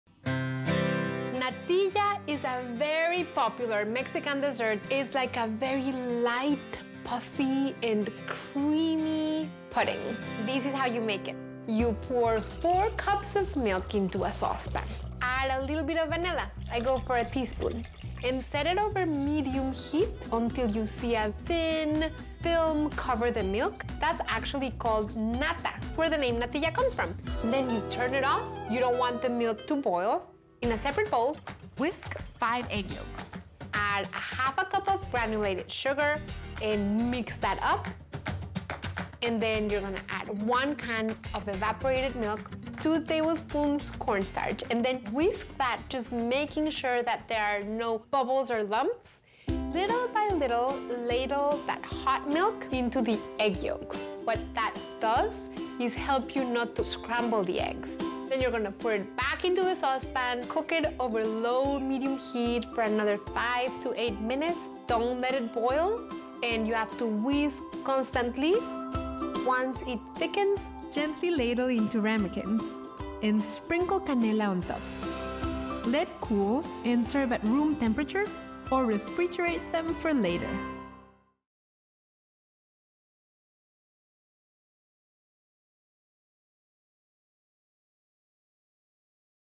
Pati Jinich - How to Make Natilla
Pati shows us how to make a very popular dessert in Mexico, called Natilla. It is a very creamy vanilla custard that can be eaten warm or cold. Her recipe is available in English and Spanish, see below...